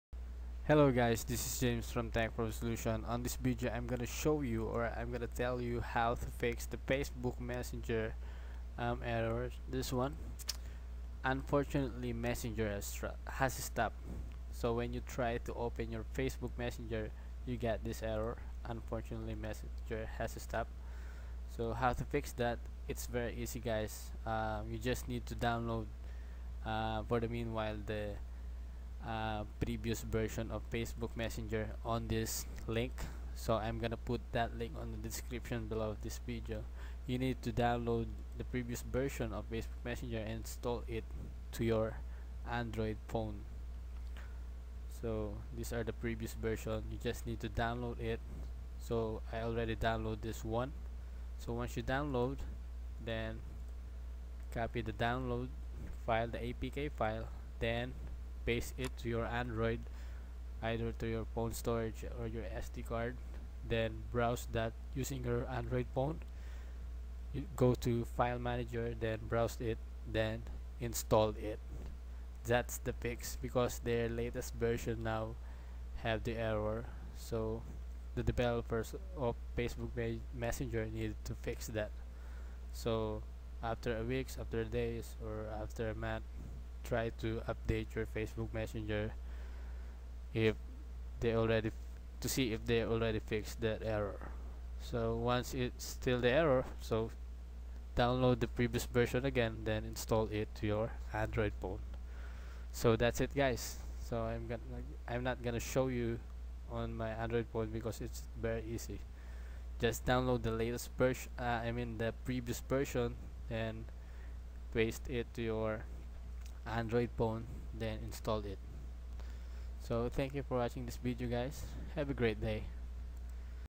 How to fix "Unfortunately, Messenger has stopped" Facebook Messenger
You can't open your Facebook messenger on your android phone and you get this error "Unfortunately, Messenger has stopped". This video will tell you how to fix that error.